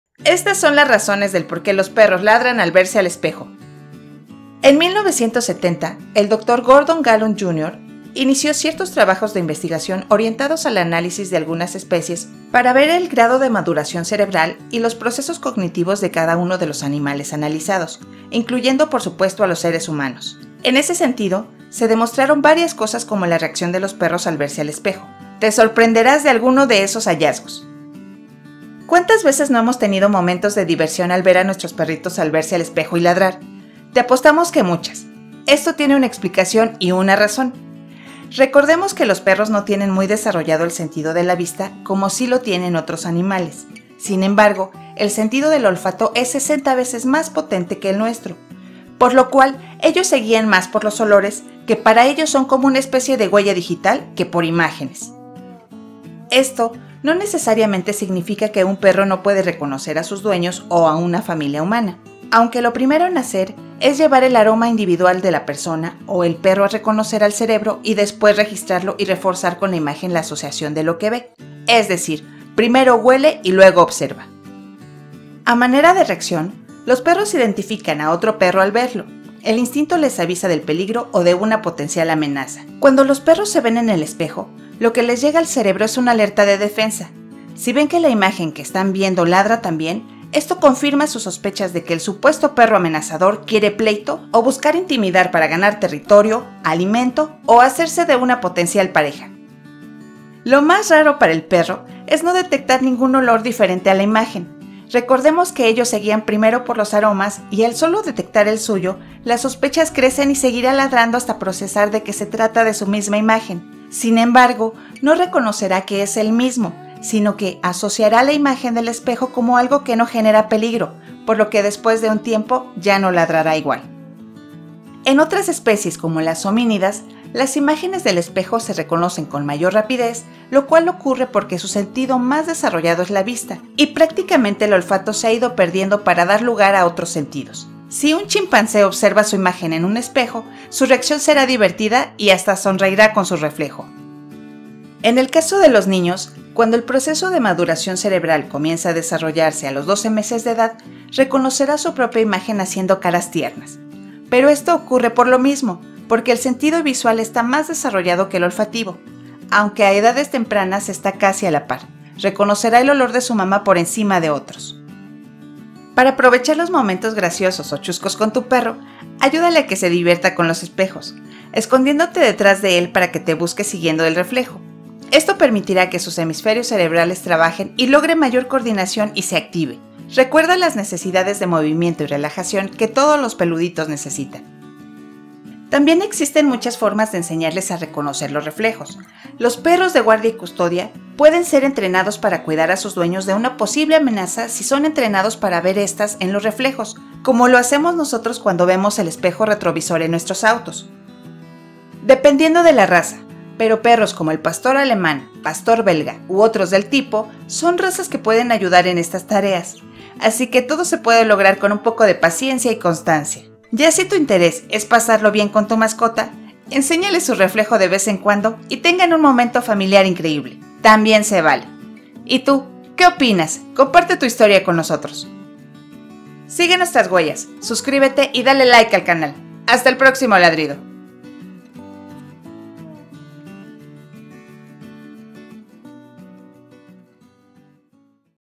Por Estas Causas los Perros Ladran al Enfrentarse al Espejo por Primera Vez
ESTAS SON LAS RAZONES DEL POR QUÉ LOS PERROS LADRAN AL VERSE AL ESPEJO. Si alguna vez has puesto a tu perrito frente al espejo por primera vez, notarás que se comporta de manera extraña. Aquí te decimos por qué y qué medidas debes tomar al respecto.